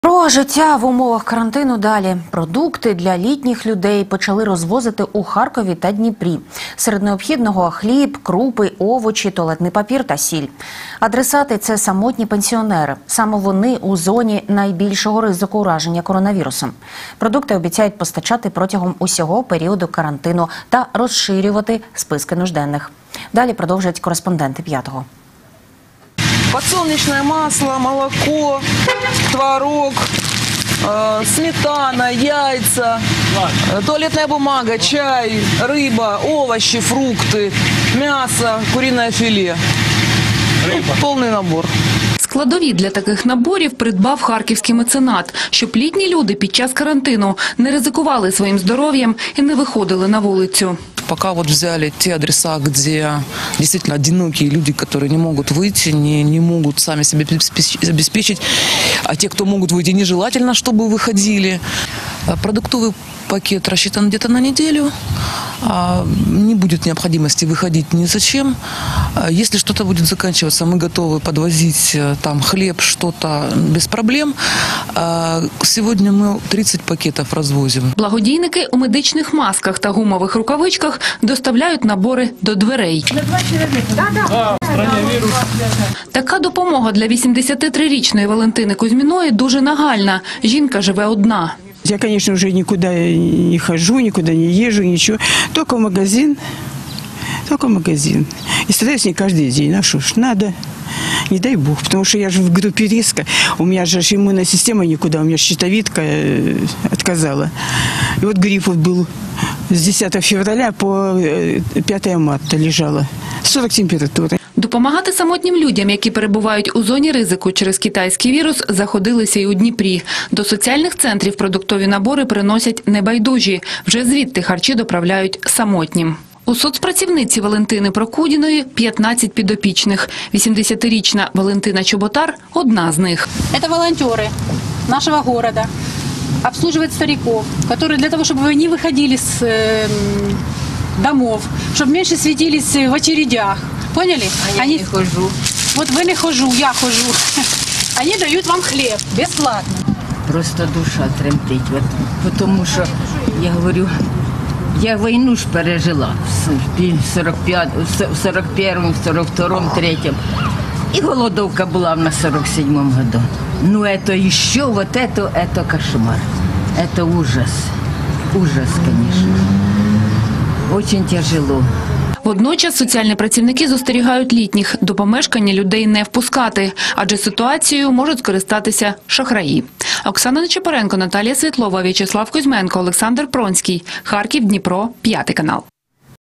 Добрі справи: продукти для літніх людей почали розвозити волонтери у Харкові та Дніпрі
Серед необхідного – хліб, крупи, овочі, туалетний папір та сіль. Адресати – це самотні пенсіонери. Саме вони у зоні найбільшого ризику ураження коронавірусом. Продукти обіцяють постачати протягом усього періоду карантину та розширювати списки нужденних.